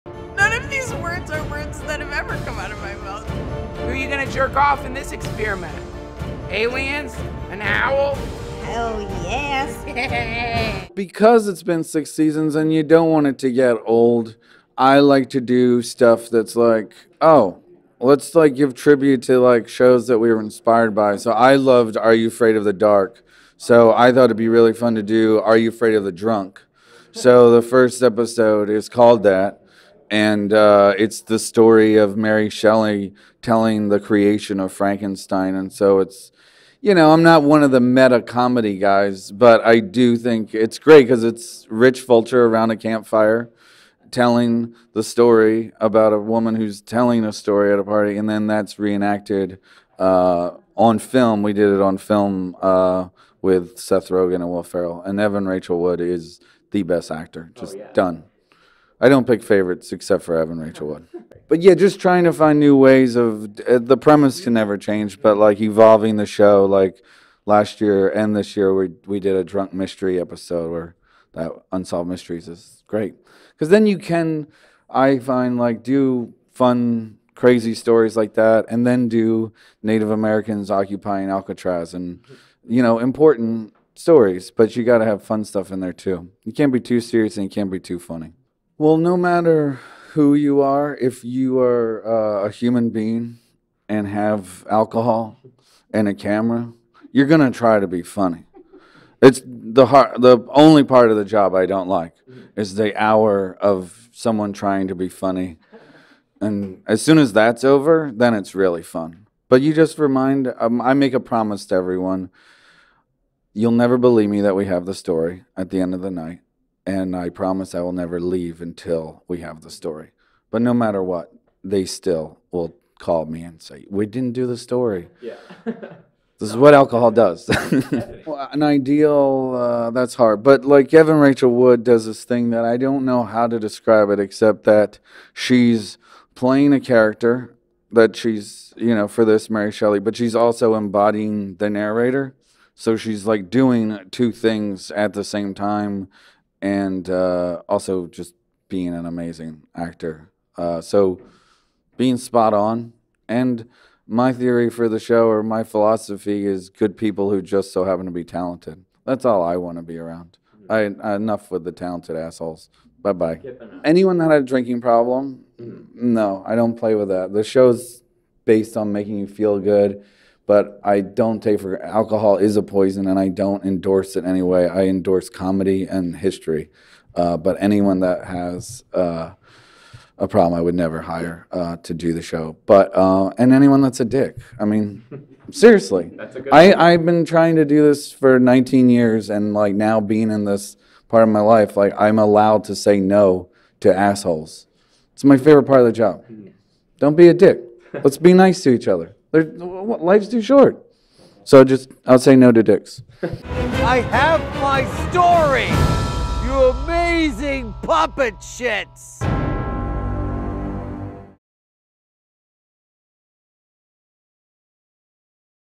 Derek Waters on the new season of Drunk History and the brilliance of Evan Rachel Wood
Drunk History returns tonight with narrator Rich Fulcher slurring his way through an Are You Afraid Of The Dark-style retelling of the creation of Mary Shelley’s Frankenstein. Ahead of the show’s premiere, we spoke with creator Derek Waters about trying to keep the show fresh after six seasons, and the brilliance of Evan Rachel Wood, who guest stars in the premiere as Mary Shelley.